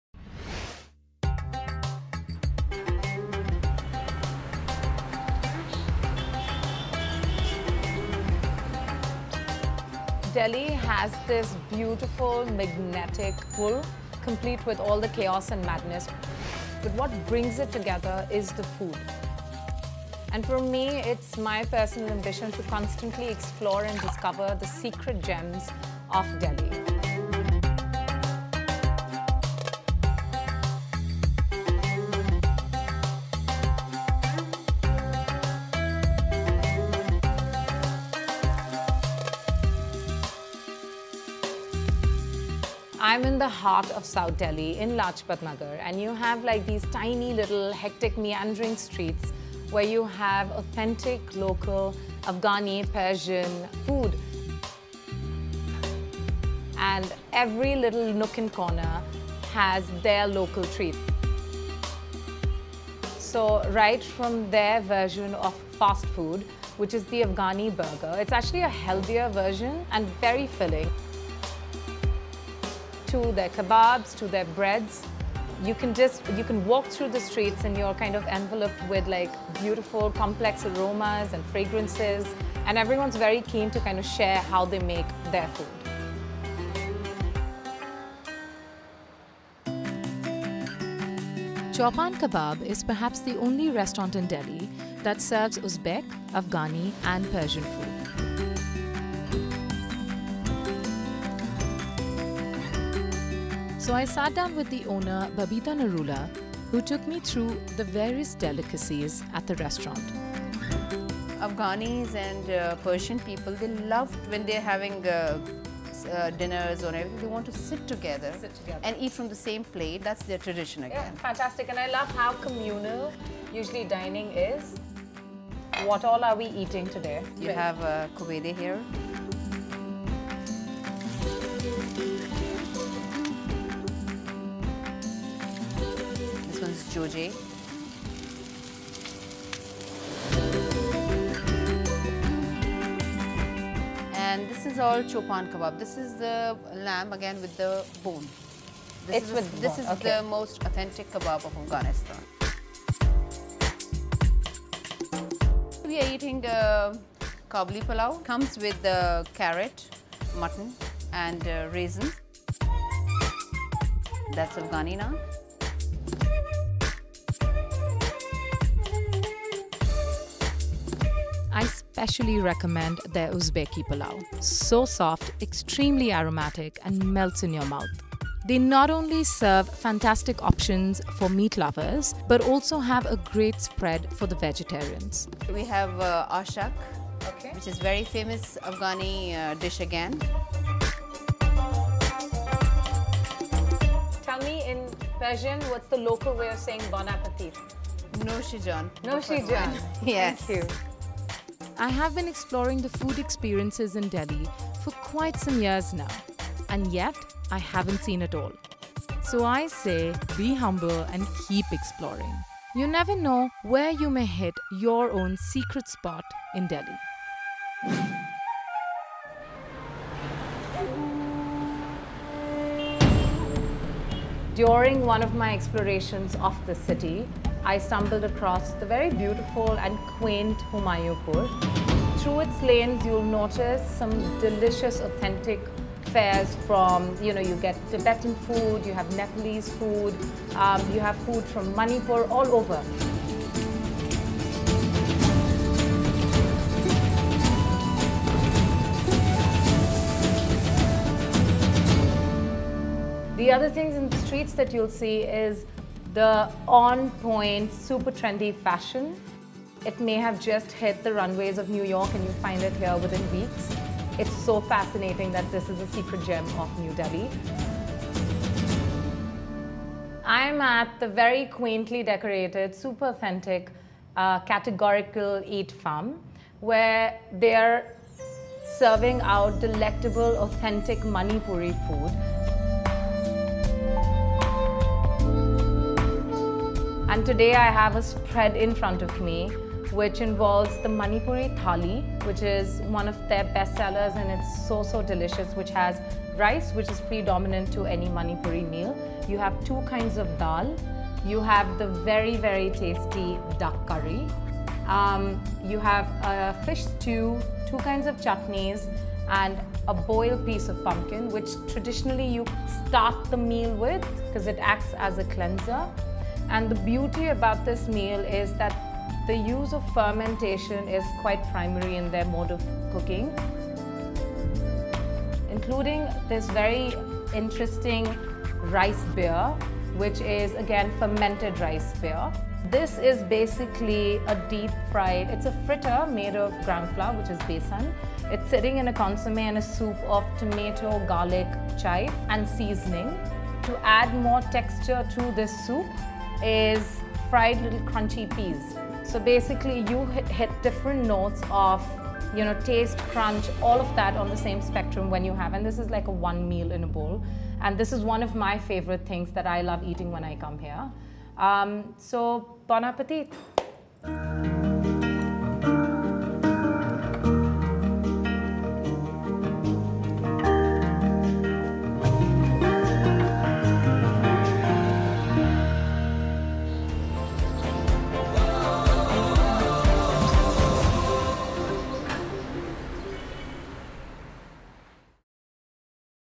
CitySecrets Delhi | Episode 2: Afghan Street & Humayunpur
A melting pot of cultures, Delhi is home to many immigrants, who bring along with them their added culture, tradition and food. Set in the bustling Lajpat Nagar is the Afghan street or ‘Little Kabul’ that serves typical Afghani fare, while on the other hand, the streets of Humayunpur offer something from every state in the North East. Housing little pockets of authentic flavours, the city brings the world on your plate.